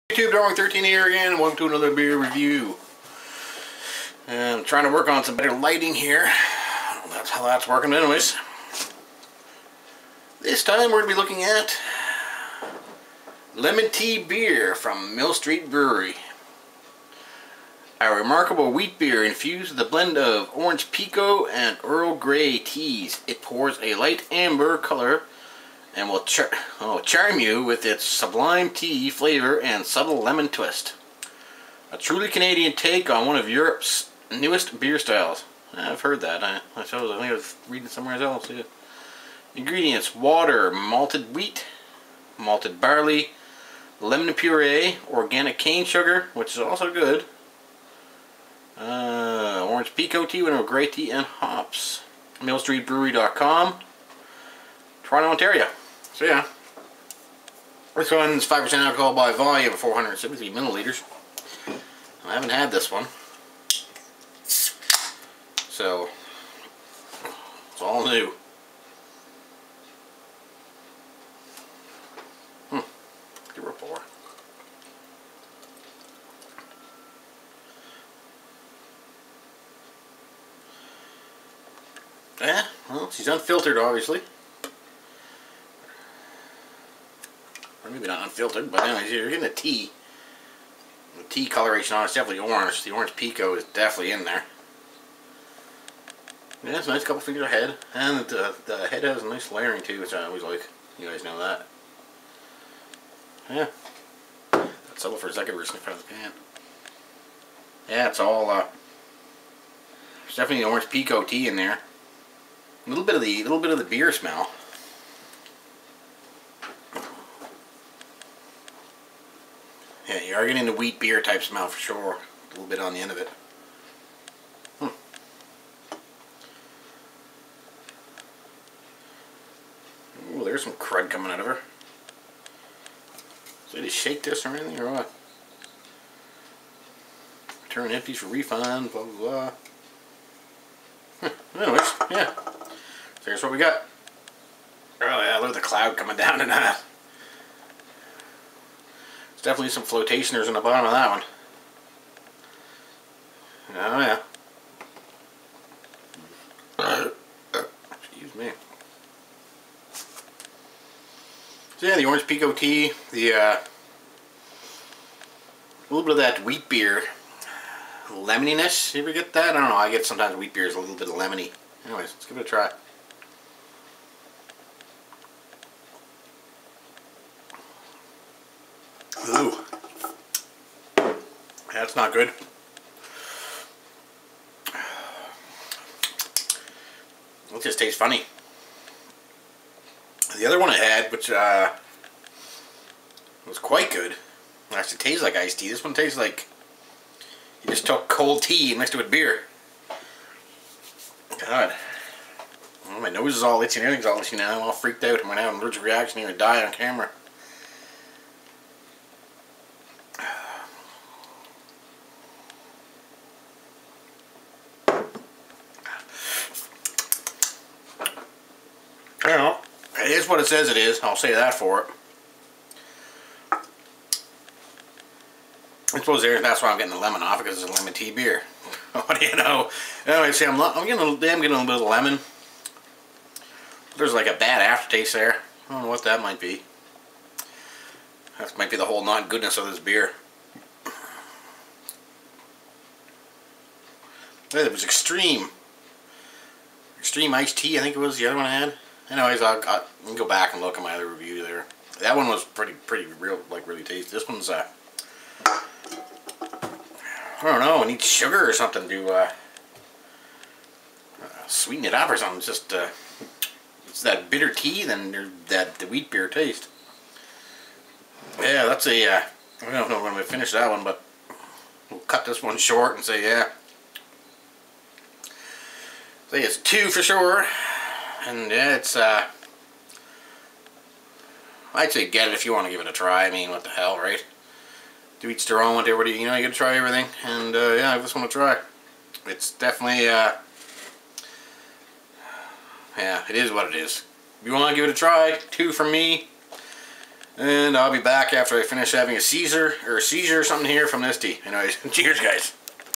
Mill St. Brewery's Lemon Tea Beer - Beer Review #96
Another flavored brew, this one from Mill St. Brewery in Toronto, Ontario...let's see how this one tasted eh!!